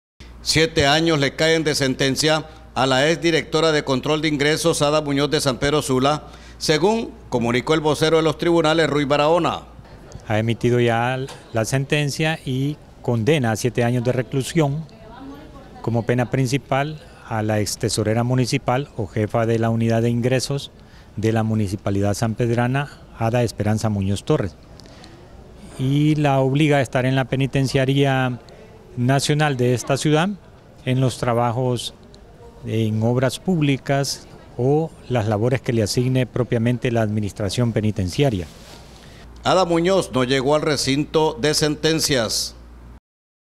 SPS SIETE AÑOS DE CARCEL PARA ADA MUÑOZ ROBERTO RODRIGUEZ
A siete  años de prisión condenan a ex tesorera de la alcaldía de San Pedro Sula, Ada Esperanza Muñoz.
El vocero del Ministerio Público en SPS informó que la acusada fue condenada por  el delito de fraude en perjuicio del Estado de Honduras.